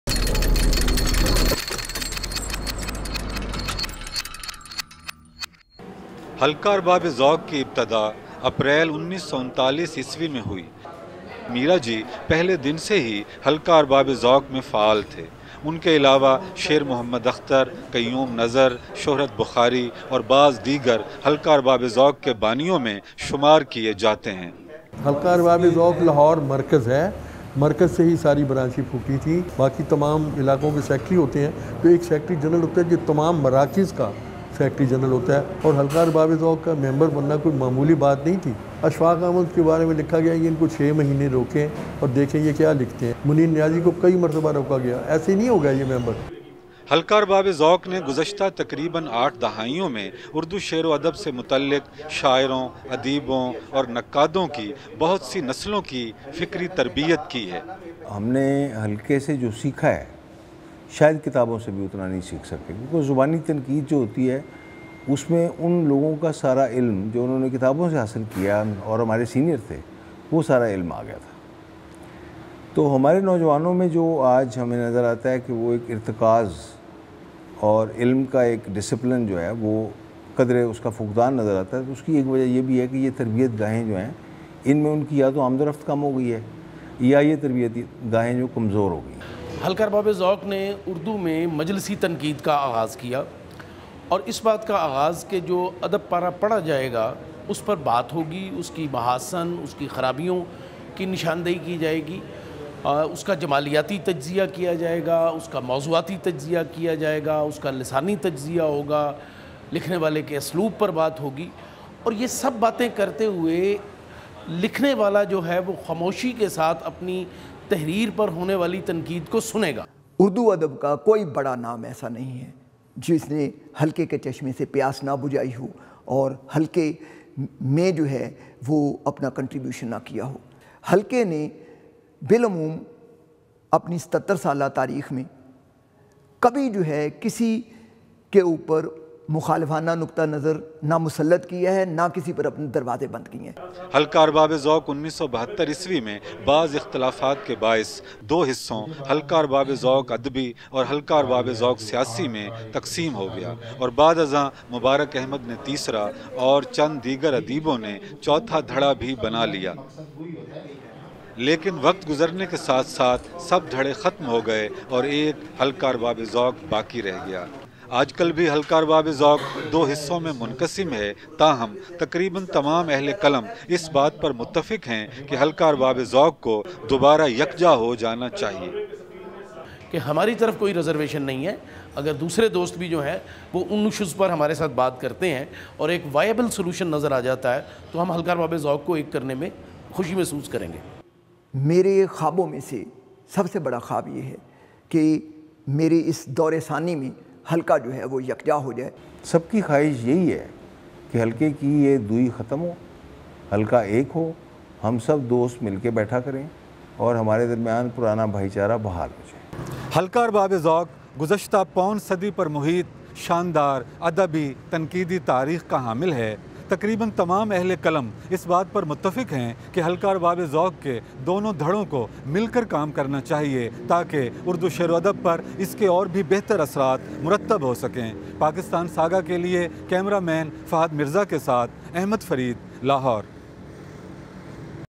حلقہ اربابِ ذوق، کیا تھا اور کیا ہو گیا
75 برس پر محیط حلقہ ارباب ذوق کا سفر آج بھی جاری ہے، مگر آج دھڑے بندیوں کے با عث اسکی تخلیقی صلاحیتیں کم ہو رہی ہیں۔ حلقے کے مثائل کیا ہیں، بتا رہے ہیں پاکستان ساگا کے نمائندہ احمد فرید۔